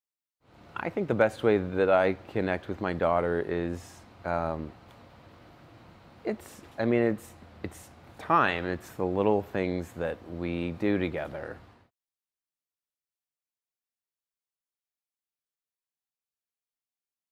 PARENTING WITH THE DOCTOR: Connecting With My Daughter
This holiday season it is so important to connect with family and friends and all those that you care about. For me, the best way to connect with the most important person in my life, my daughter, is through spending time with her. Cliché yes, but so necessary. Every moment counts with her. Who will you be spending your holidays with and what do you do to celebrate your time together?

Dr. Larry Burchett, ER Doctor and Author of "The Gentleman's Diet"